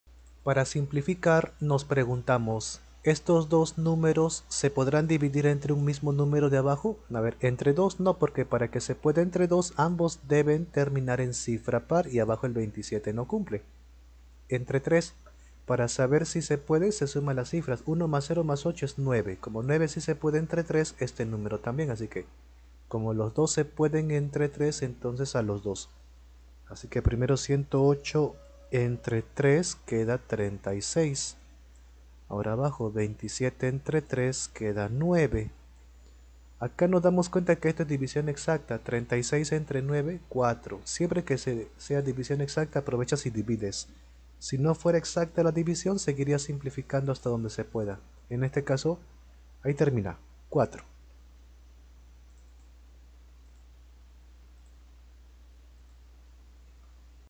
Simplificar 108/27 reducir a su minima expresion irreducible, simplificacion de fracciones
reducir fracciones a su minima expresion
hallar la fraccion irreducible